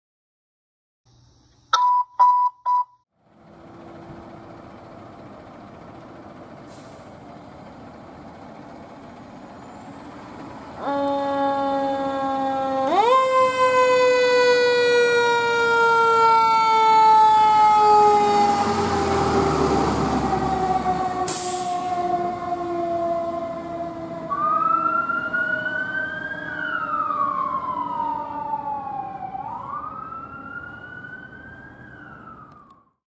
Manhattan Beach Fire Dept. Engine 22 Responding
Engine 22 is seen here responding to a medical call.

Manhattan Beach, CA - January 2023